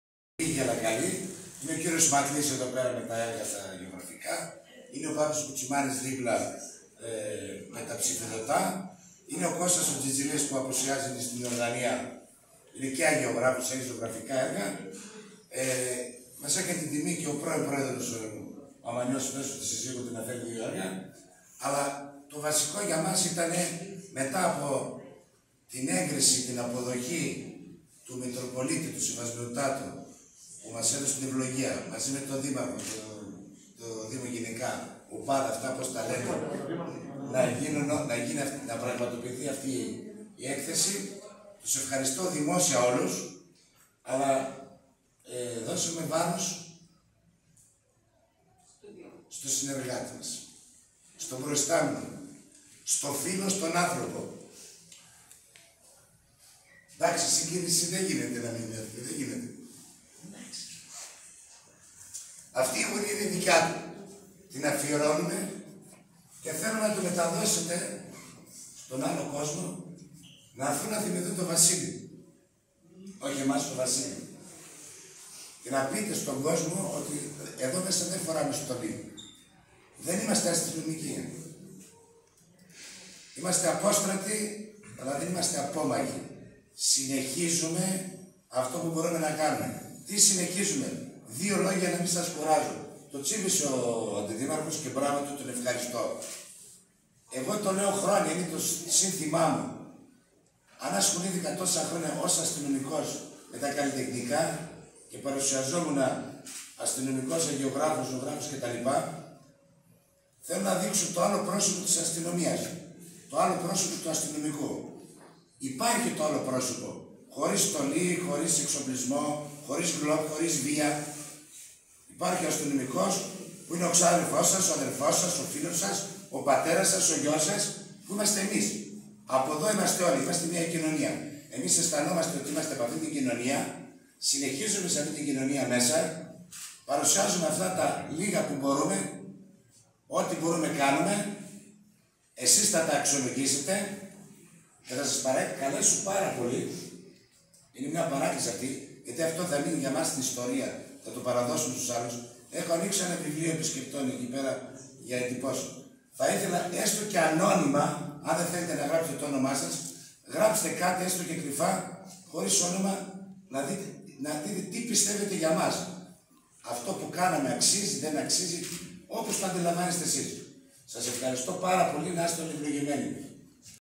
Απόστρατοι αστυνομικοί καλλιτεχνούν & παρουσιάζουν έργα τους στα «Αικατερίνεια 2021»
Χαιρετισμός και αναφορά στο περιεχόμενο και το σκοπό της Έκθεσης από τον Δέλλα Χρήστο, απόστρατο αστυνομικό και δημιουργό, μέλος του Δ.Σ. (Ταμία) του του Συνδέσμου Αποστράτων Σωμάτων Ασφαλείας Ν. Πιερίας

Δεν υπήρξαν μόνο φρουροί του νόμου και της κοινωνίας, ήταν (και παραμένουν) αξιόλογοι εικαστικοί καλλιτέχνες. Ο λόγος, για αστυνομικούς της Πιερίας που όταν ήταν εν ενεργεία, παράλληλα με τα υπεύθυνα καθήκοντά τους, καλλιτεχνούσαν και το ίδιο συνεχίζουν να κάνουν έως και σήμερα ως απόστρατοι. Πρόκειται για τους Κωνσταντίνο Τζιτζιλή, Μπάμπη Κουτσιμάνη, Γεώργιο Ματλή και Χρήστο Δέλλα. Η έκθεση είναι αφιερωμένη στον αείμνηστο, Βασίλης Κιο, ο οποίος διετέλεσε Διευθυντής Ασφαλείας της Διεύθυνσης Αστυνομίας Πιερίας, καθώς και Αστυνομικός Διευθυντής Μαγνησίας. Εκτίθεται και έργα που δημιούργησε ο εκλιπών, απόδίδοντας του έτσι τιμή εκ μέρους των διοργανωτών αποστράτων αστυνομικών Πιερίας